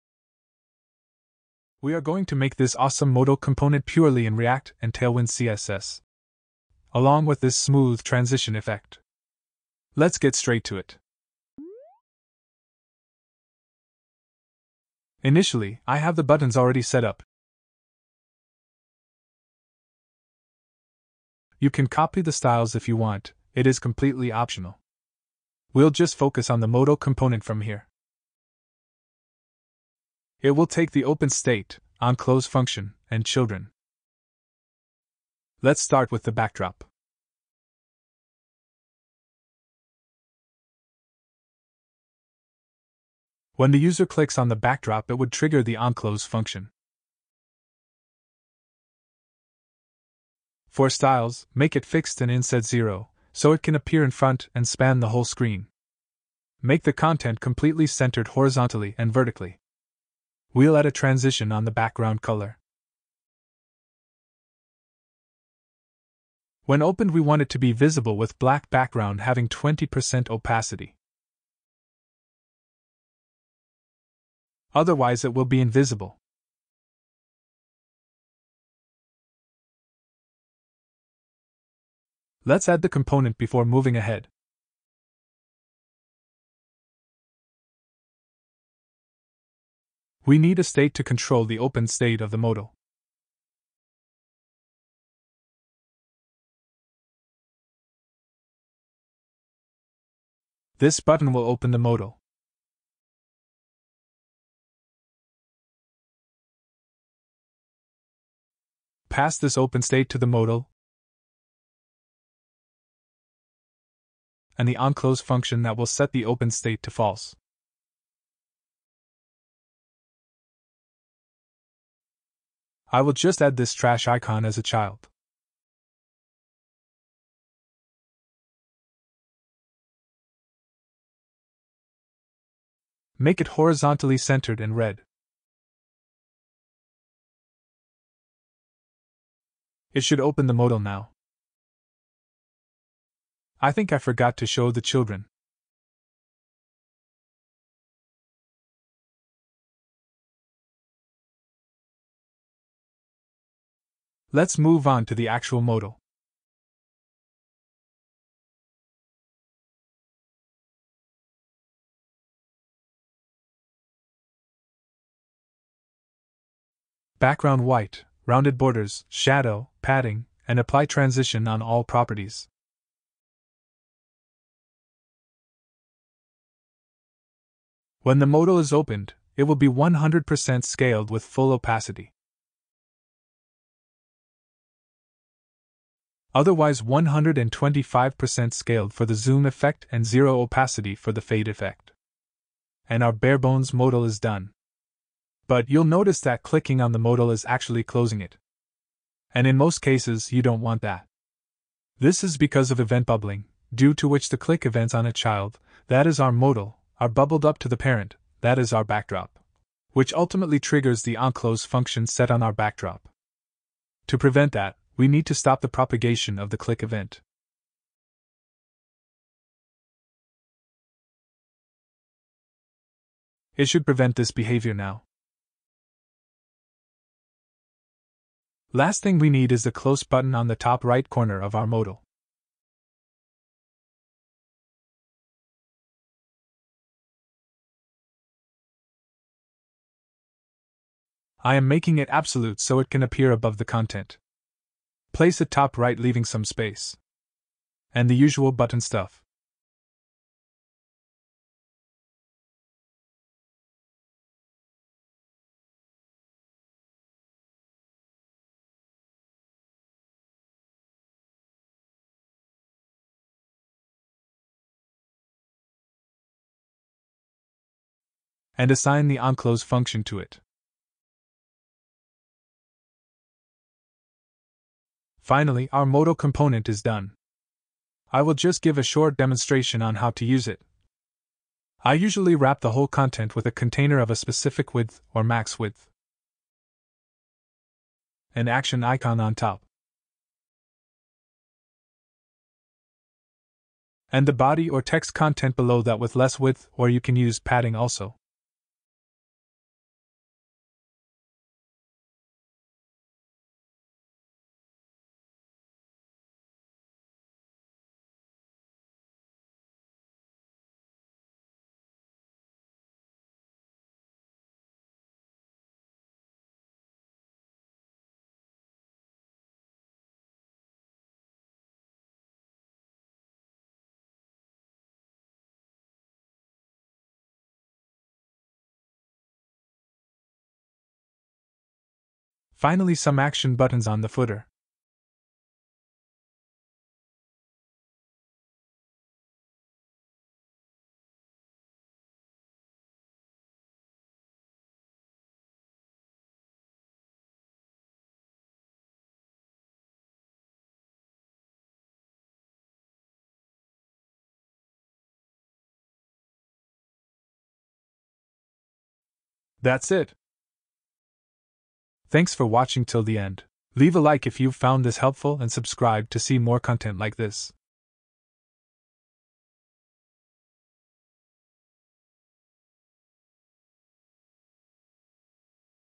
Build a Modal Component purely in ReactJS and TailwindCSS
In this video you'll learn how to make a Modal or Dialog Box Component in ReactJS and Tailwind CSS from scratch (without any additional libraries) under 6 minutes, which will be a reusable and customizable component that you can use in your own projects.  We will also cover how to add smooth transitions to the Modal Component easily only using Tailwind CSS.

A Modal Component is a common UI element used in many web applications to display content in a popup window. By the end of this video, you will have a working Modal Component that you can use in your ReactJS projects. Whether you are a beginner or an experienced ReactJS developer, you will find value in this tutorial.